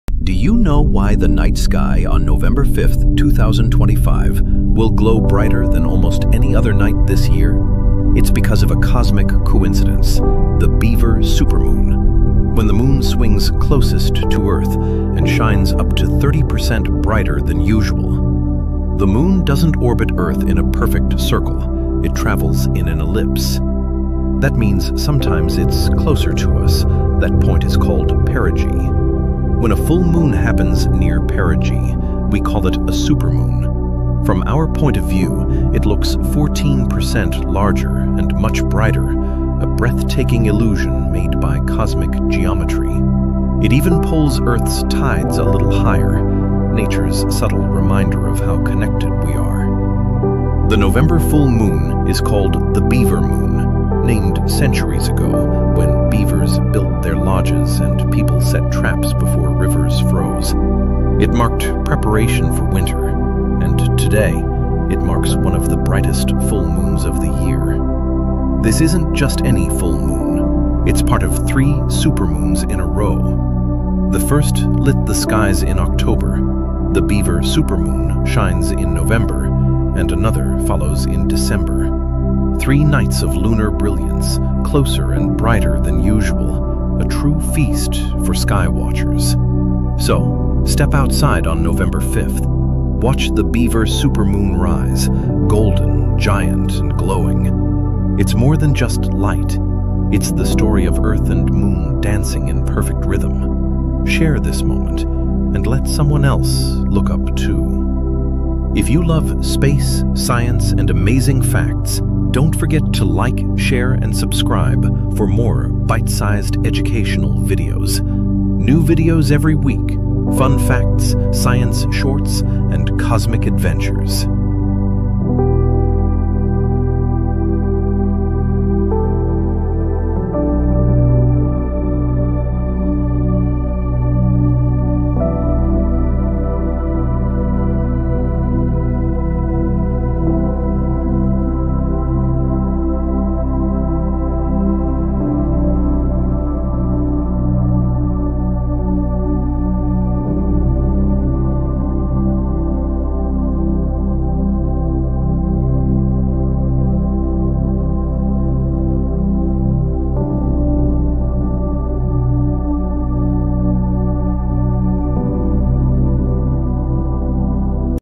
What Makes The Beaver Supermoon So Special This Year?
Did you know the November moon brings a unique full moon experience? 🌕  On November 5th, 2025, catch the supermoon November, a rare celestial events when the interplay of gravity, physics, and the solar system align to make the Moon appear brighter and closer than usual! Have you ever wondered why the night sky looks extra bright on certain full moons? 🌕 On November 5th, 2025, Earth will witness an incredible celestial event — the Beaver Supermoon, one of the brightest and closest full moons of the year!

In this fascinating short, we explore the science behind the Supermoon — how the Moon’s elliptical orbit, perigee, and Earth’s gravitational pull align perfectly to create a Moon that shines up to 30% brighter and 14% larger than usual. You’ll also learn the origin of the “Beaver Moon” name, rooted in ancient traditions that marked the start of winter preparations.

This episode of “Do You Know?” blends space facts, science education, and relaxing ambient music to spark curiosity and inspire wonder about our cosmic neighbourhood. Perfect for stargazers, science lovers, teachers, and anyone who enjoys discovering how our Moon and Earth dance together in orbit.